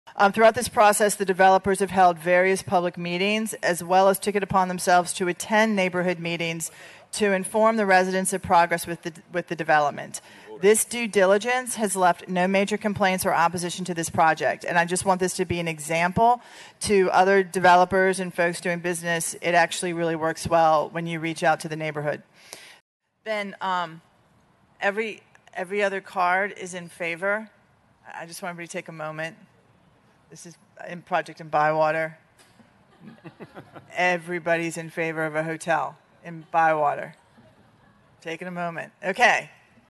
Bywater Hotel Council Meeting
After an early, transparent, successful community engagement process, a new hotel development in an historical neighborhood passed in City Council without any negative response.